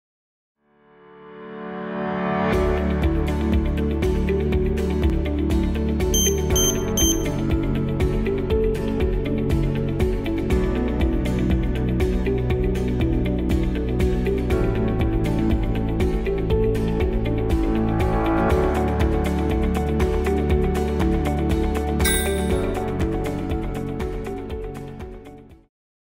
フルカバーホースリール（20M） 巻き取り・ホースの汚れ落としが簡単
フルカバーホースリール（20M）の巻き取りやすさ・汚れ落としの簡単さを動画で紹介